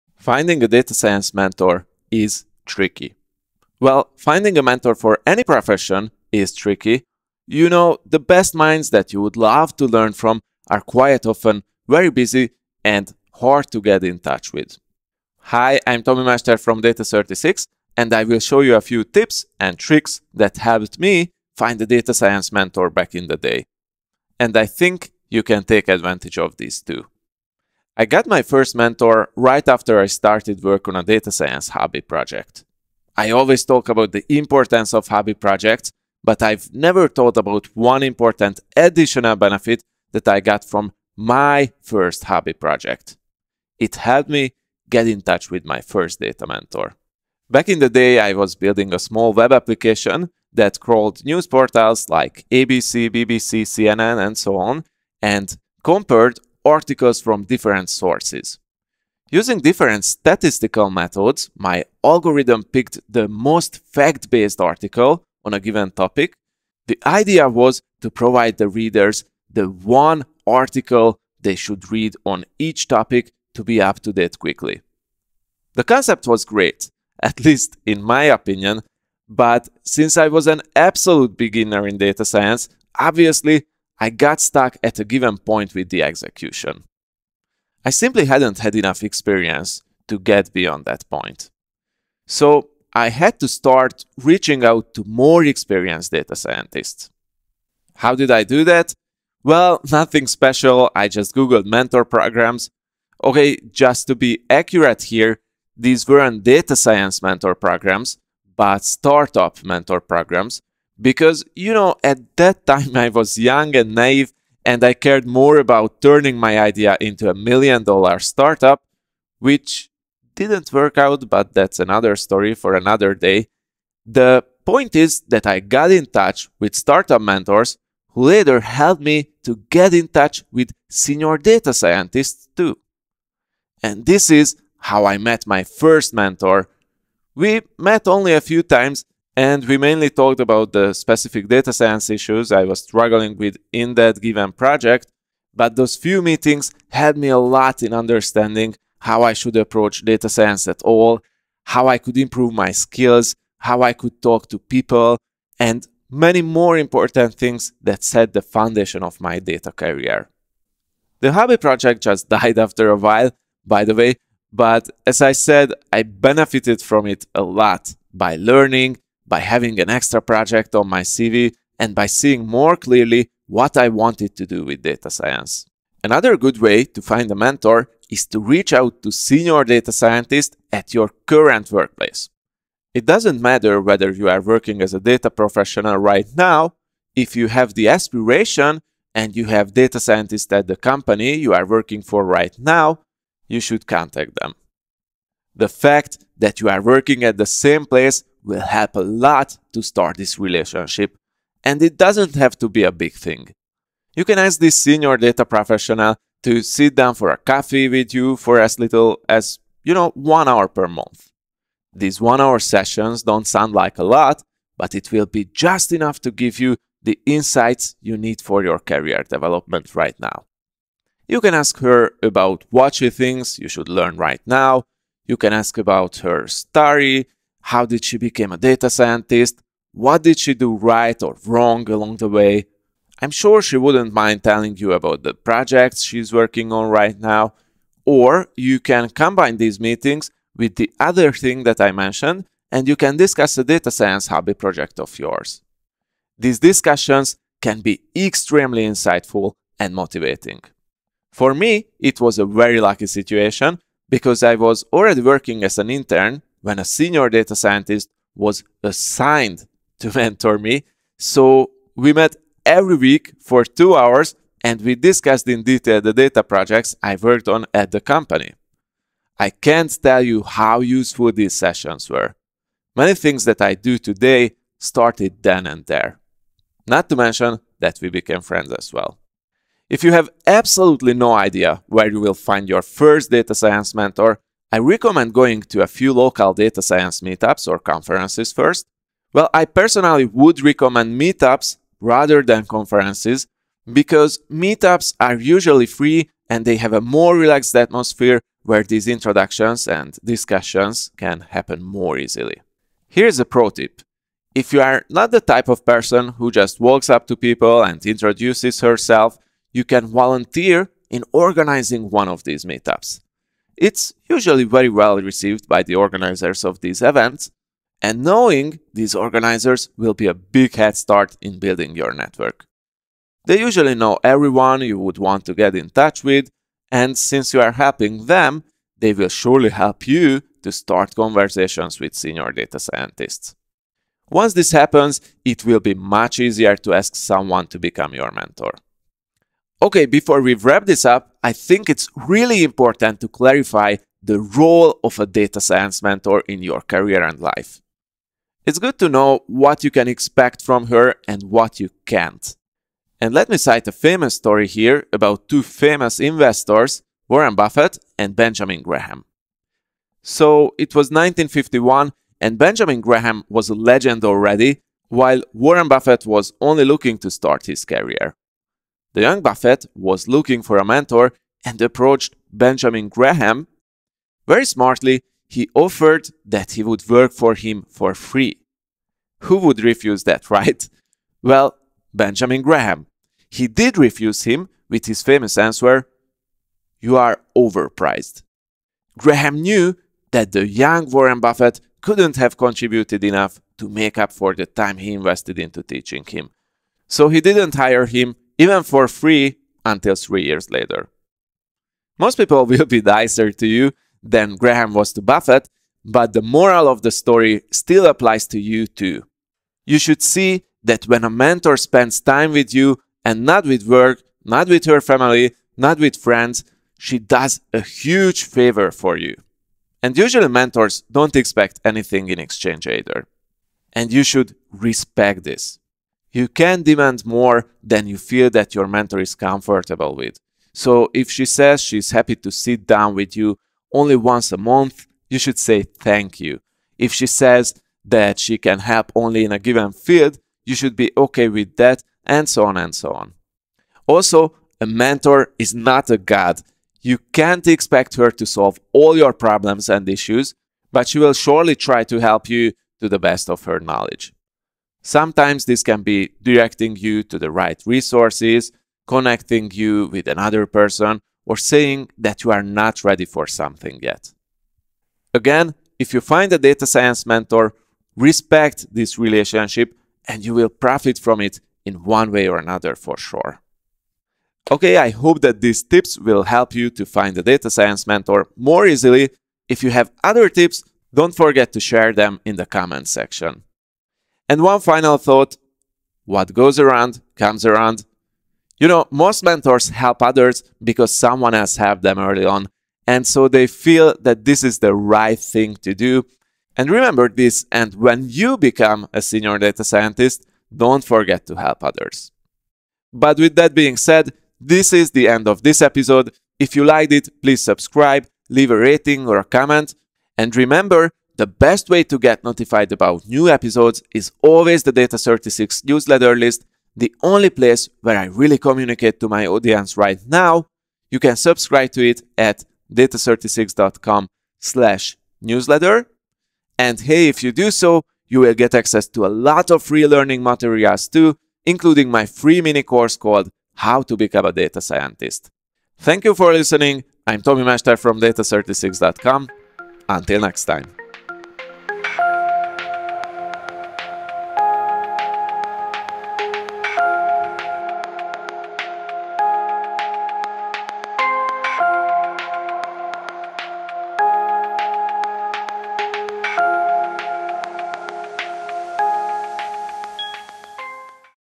How to Find a Data Science Mentor (3 Tips)
Finding a data science mentor is tricky… Well, finding a mentor for any profession is tricky!

You know, the best minds that you would love to learn from are quite often very busy and hard to get in touch with. In this article, I’ll show you a few tips and tricks that helped me find a data science mentor back in the day — and I think you can take advantage of these, too.

00:00 Intro
00:27 Start a hobby project and reach out
02:55 Find it at your current job
04:40 Go to local data science meetups or conferences!
05:39 What to expect from a data science mentor
07:54 Conclusion

LINKS MENTIONED IN THE EPISODE:
- Meetup.com

IMAGE SOURCES:
- wikipedia